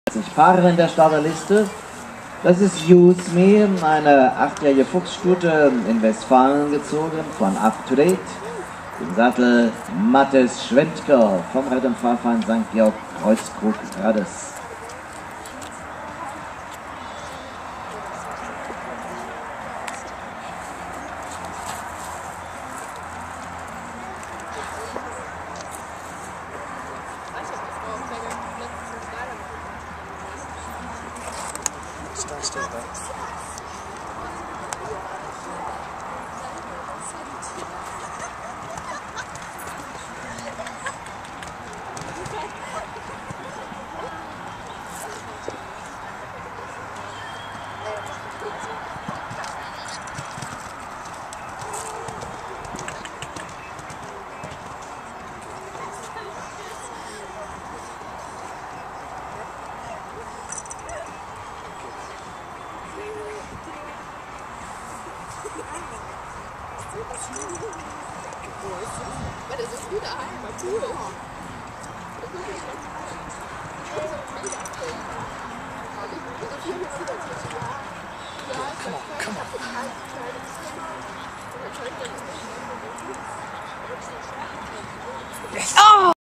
Use Me and Mathis Schwentke Mannheim 2011
GFH Stud's Use Me jumps in her first International 1.40 class and just had the 'Joker' fence (1.45!) down. Mannheim Maimarkt Turnier 2011. For more current news and information please visit our facebook